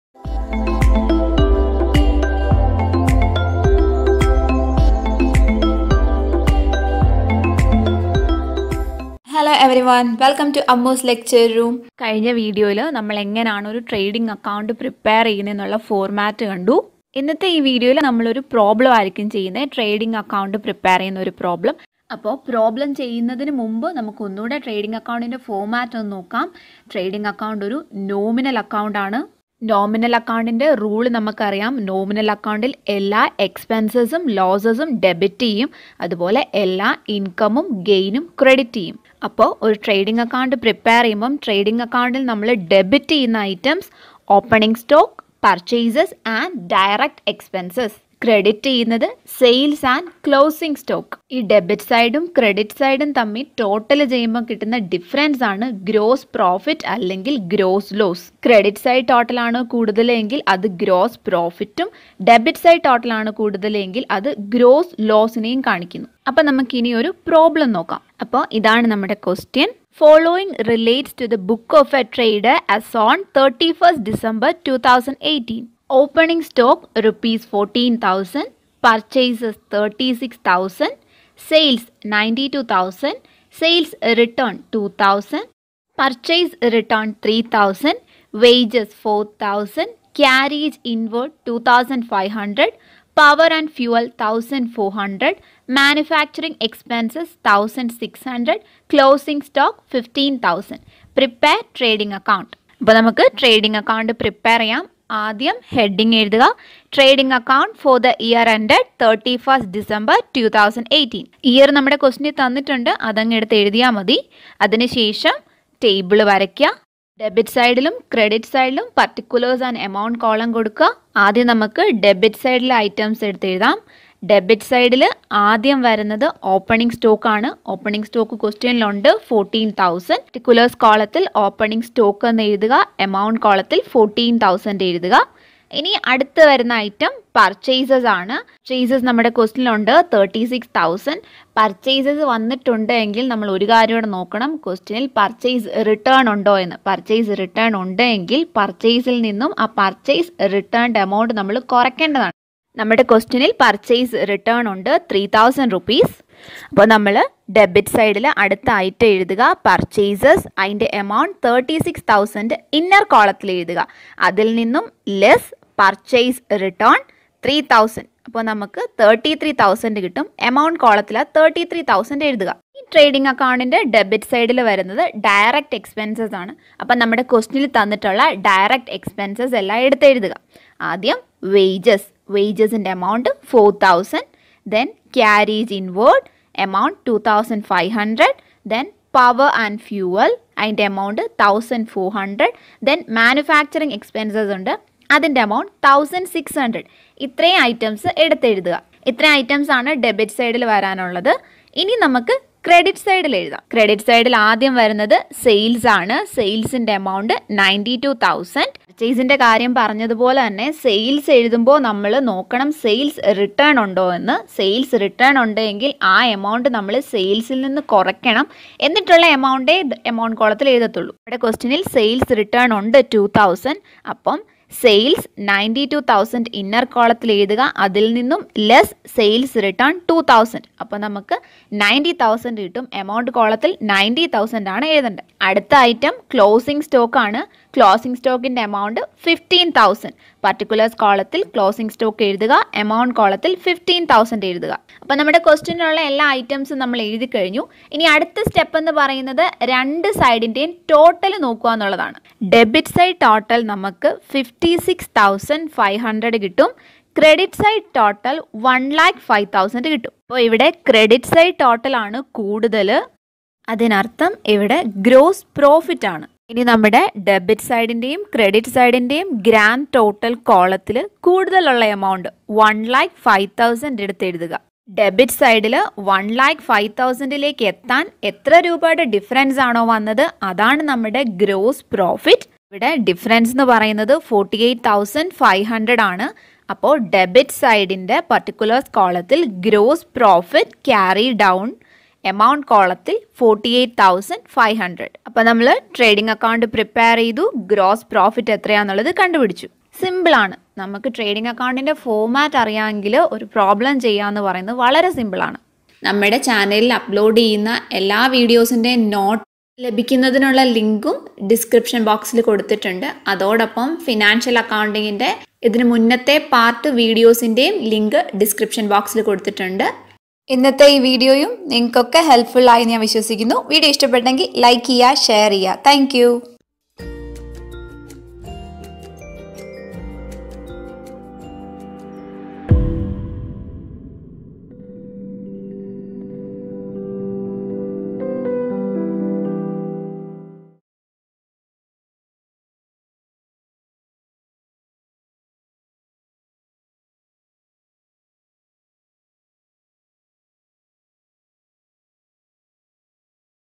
PART 08: FINANCIAL ACCOUNTING - Trading Account (problem solving)
Let's Study Commerce - Classes in Malayalam

PART 08: FINANCIAL ACCOUNTING - Trading Account